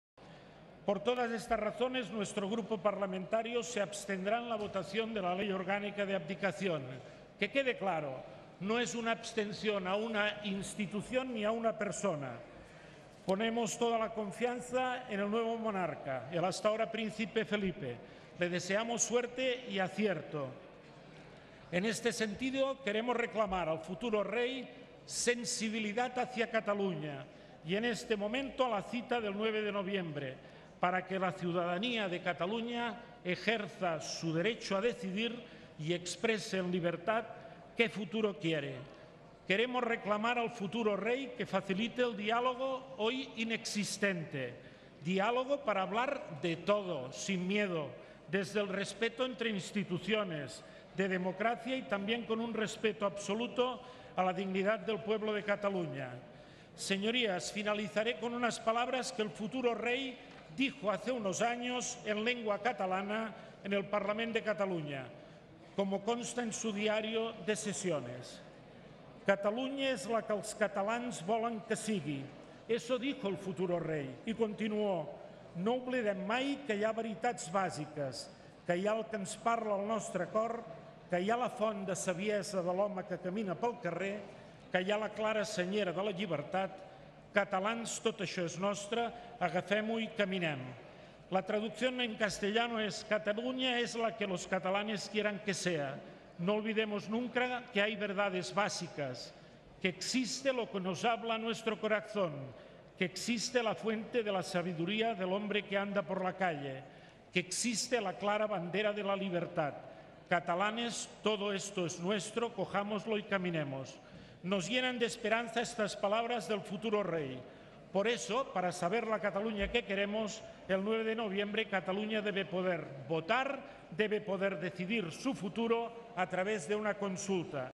CiU reclama a Felipe VI "diálogo" y "sensibilidad"
El portavoz de CiU en el Senado, Josep Lluís Cleries, ha manifestado este martes en el Senado durante el debate de la ley de abdicación que su formación confía en que el futuro Rey Felipe VI contribuya "a desbloquear diálogos" para que los catalanes puedan decidir su futuro "mediante la consulta que tendrá lugar el 9 de noviembre".